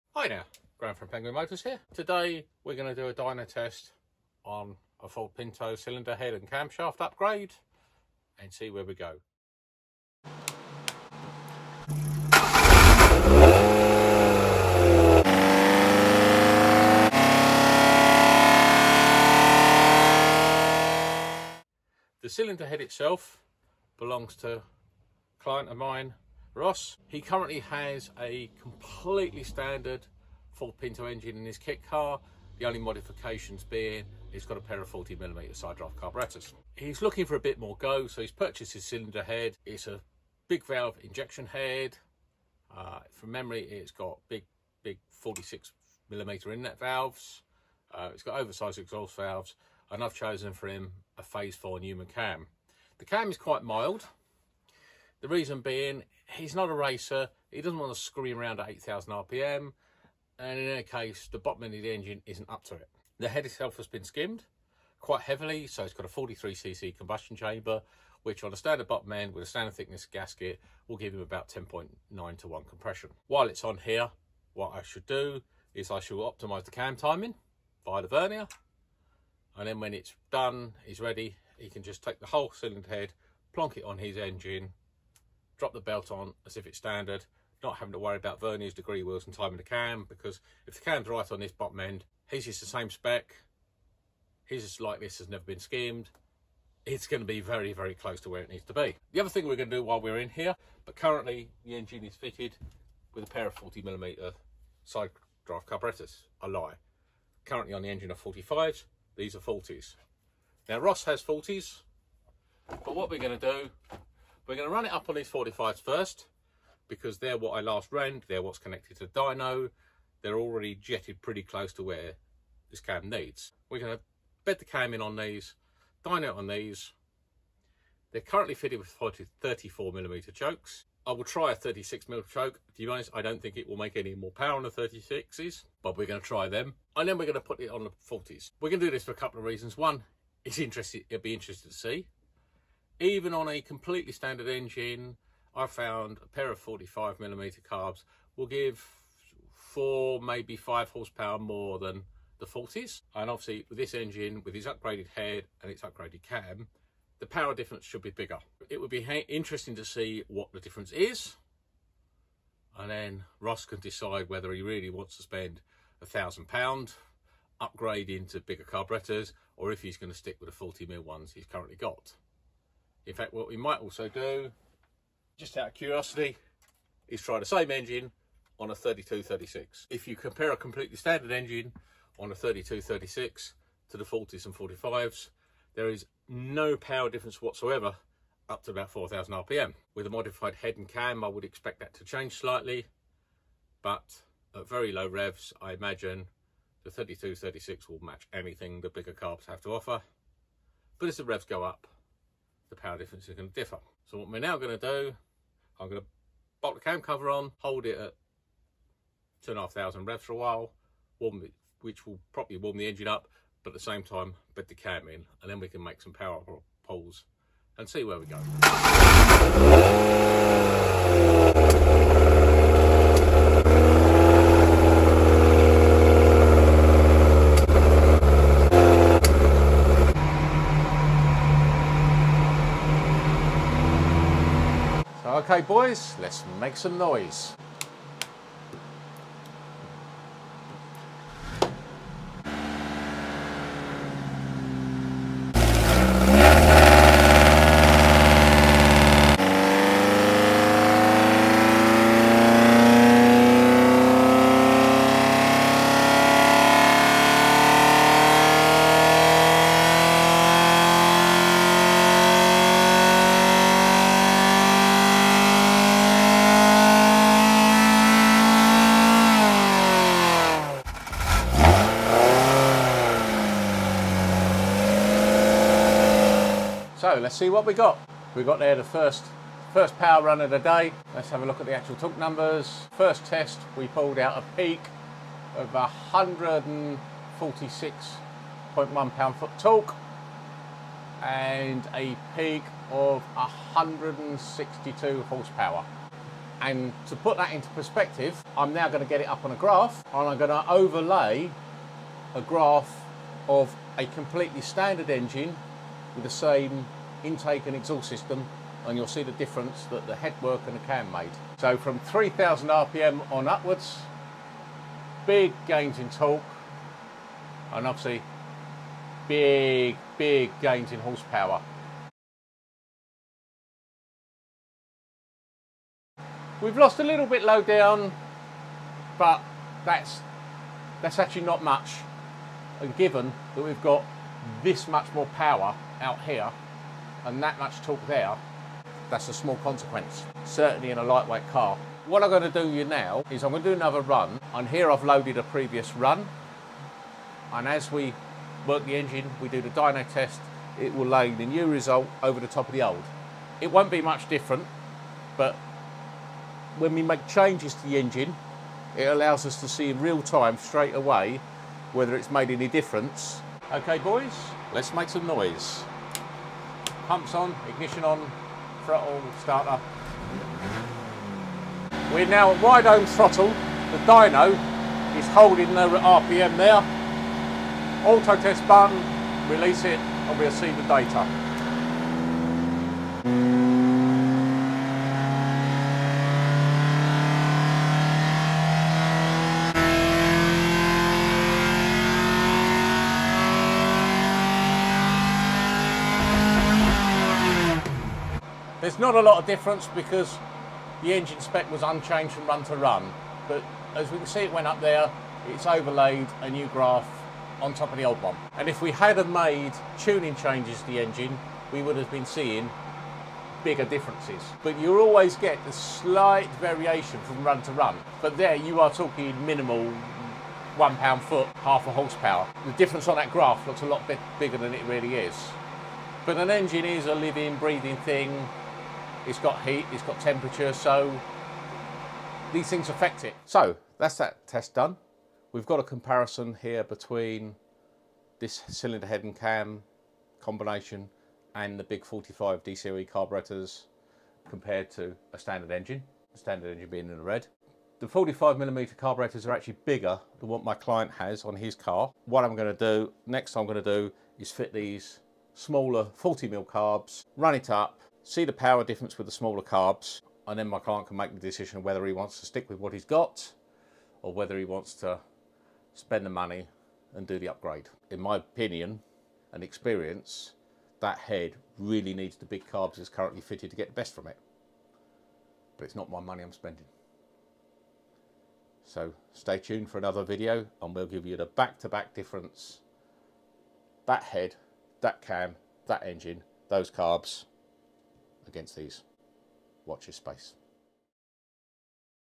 Ford Pinto 2.0 Big Valve & PH4 Newman Cam Vs Standard Head/Cam Dyno tested
This Standard Ford Pinto 2.0, 45mm  DCOE sidedraught carburettors, to be upgraded to:
Big valve injection head, 46mm inlet valves, oversized exhaust valves, Phase 4 Newman Cam.

Ford Pinto Cylinder Head and Cam upgrade

Future works: Dyno dog engine currently fitted with 45mm sidedraught carburettors, will be testing with 40mm sidedraught carburettors for direct comparison. Should I upgrade? Is it worth upgrading for the £1,000 price mark? Will bring you a future video to discuss just this!

How much horsepower does a Ford Pinto have? We run it on the Penguin Motors Superflow Dyno to find out!
What engine was in the Ford Pinto? Graham explains all in this video!